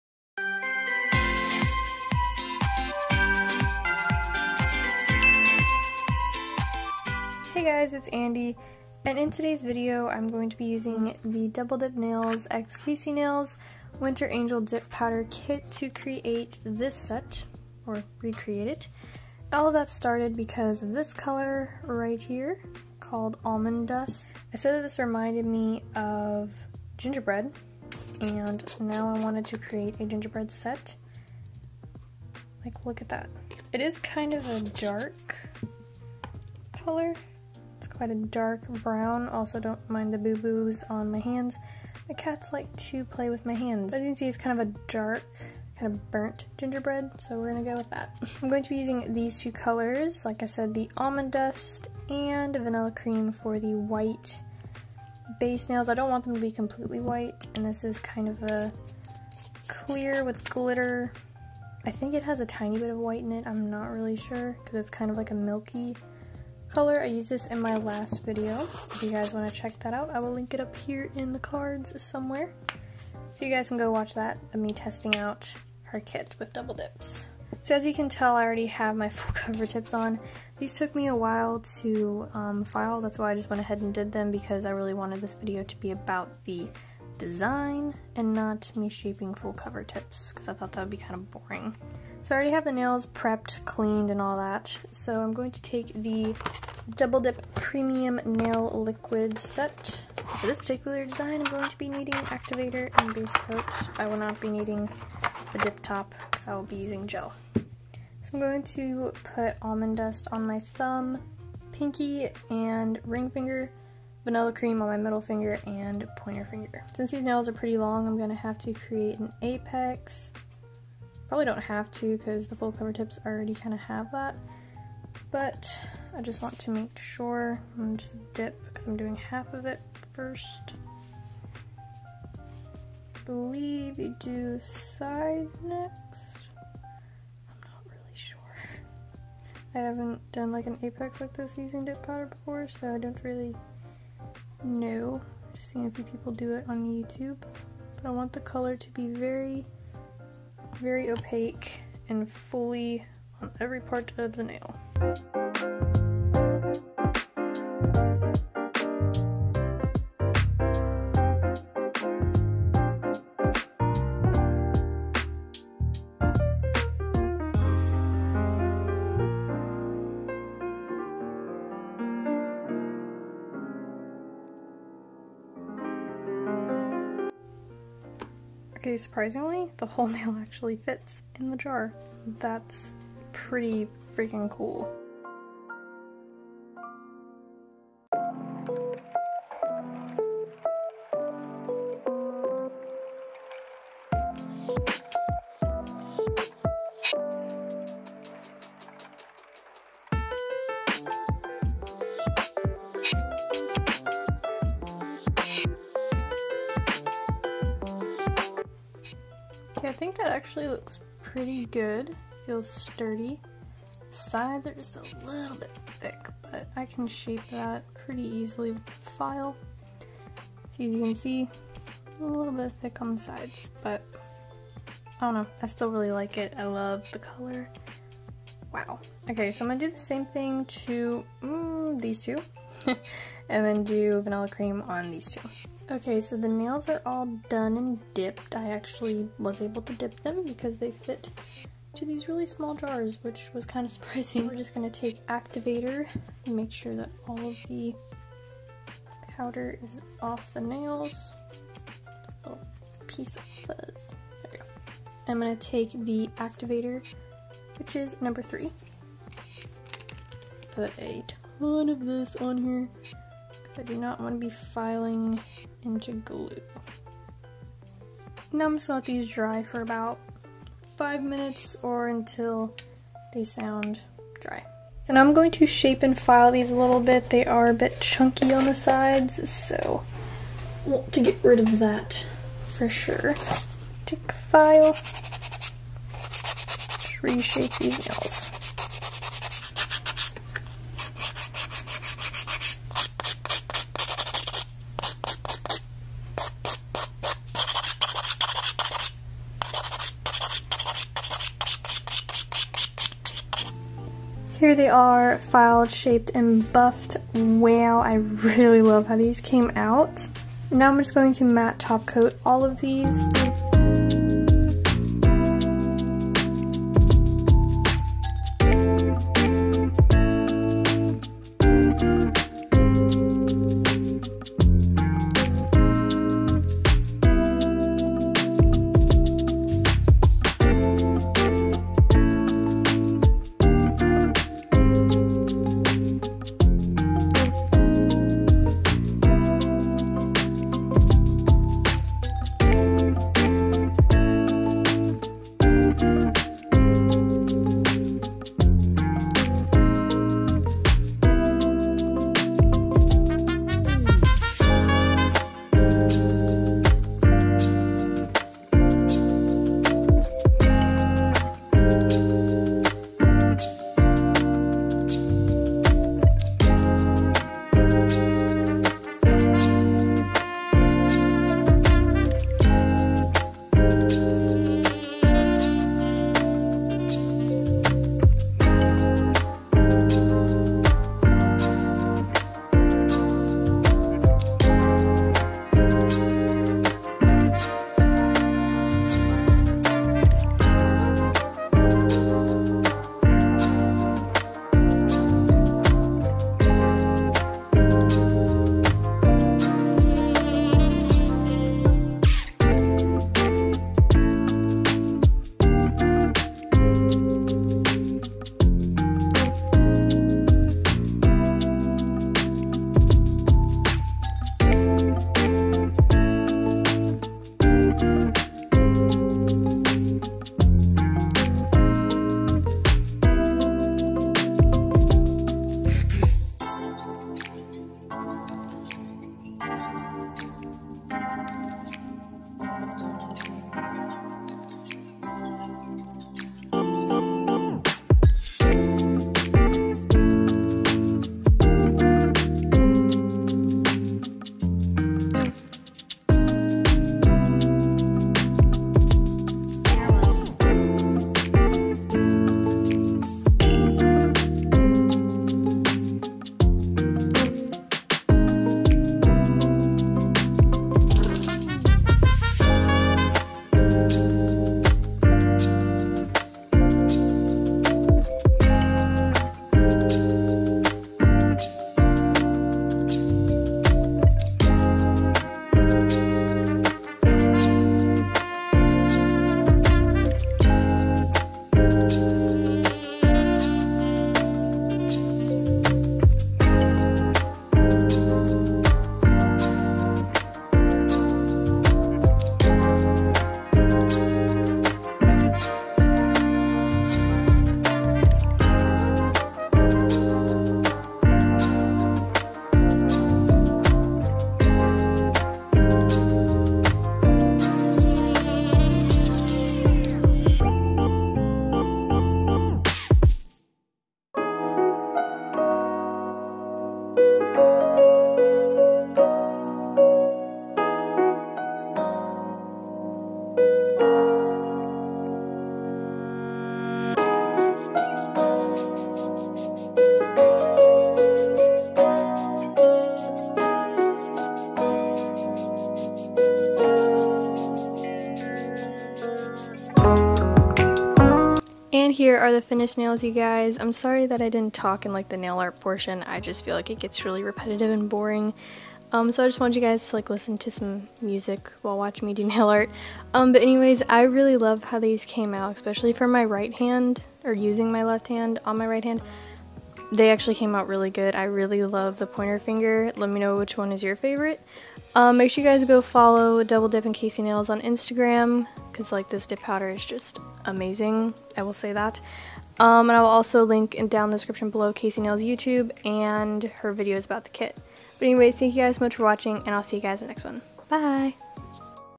Gingerbread Man Nail Set + XL Nails With Dip Powder | Tutorial
hope y'all enjoyed!

hey y'all! today I am using the double dips x KC nails limited addition (winter angel) dip powder kit! but this time I will be doing some LONG nails! these were so so fun to recreate and I hope y'all like it! make sure to like and subscribe for more videos! also make sure to use Kate's code at checkout (kcnails20) to save some money with your order!

makartt matte top coat-

gel polish

nail lamp

makartt full cover nails

makartt black and white gel- polish

nail gems

*I take no credit for what I do, God helps me tremendously!*